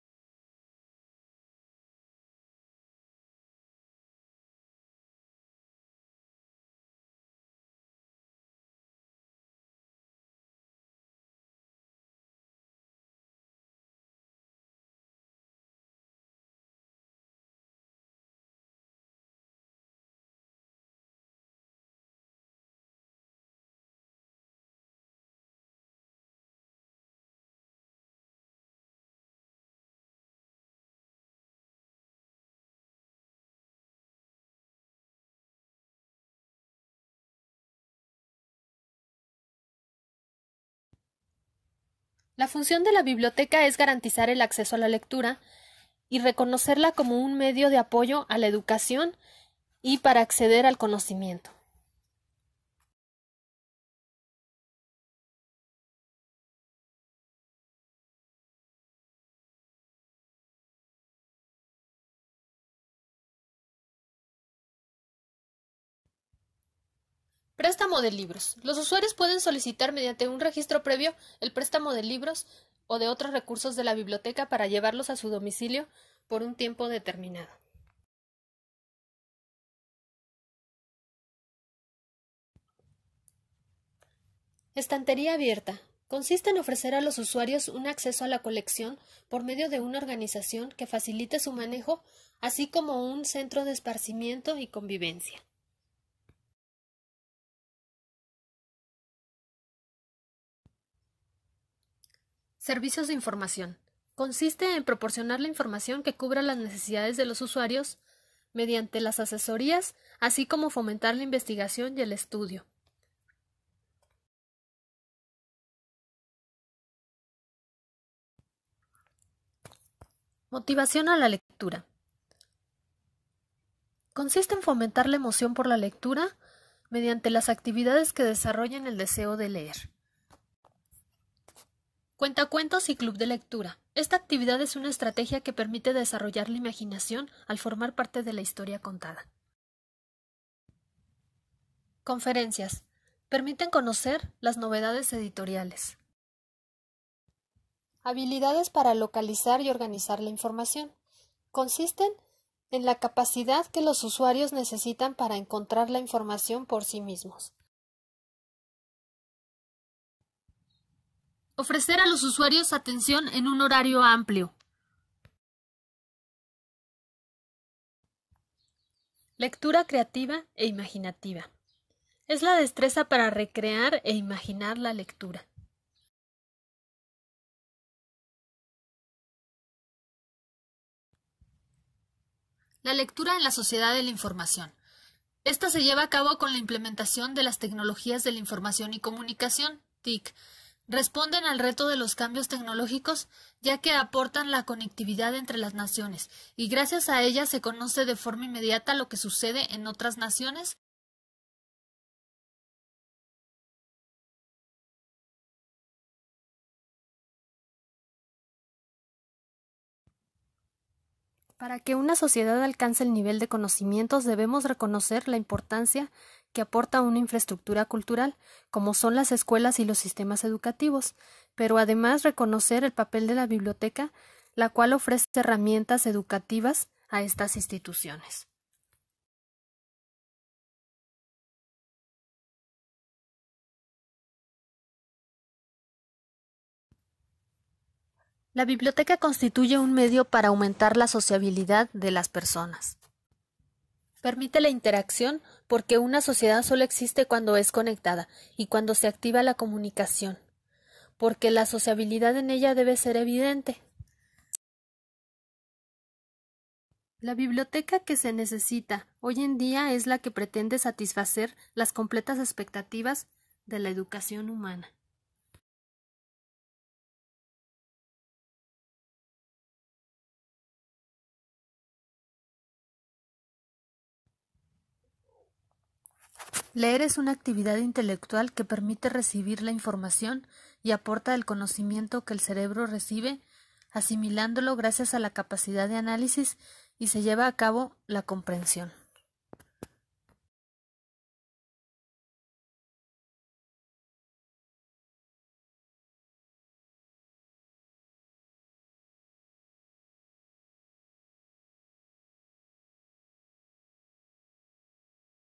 Biblioteca y lectura
Video que explica el papel que juega la biblioteca como medio de comunicación mediante el uso de  programas para el fomento a la lectura.

Autor:
Natividad Camacho, Sujei Milagros.
Facultad de Filosofía y letras, SUAyED.

Recurso desarrollado para el proyecto PAPIME PE403219  “Materiales didácticos para la enseñanza de la bibliotecología a distancia del SUAYED” con el apoyo del programa UNAM-DGAPA-PAPIME.

Licencia:
CC-BY-NC- SA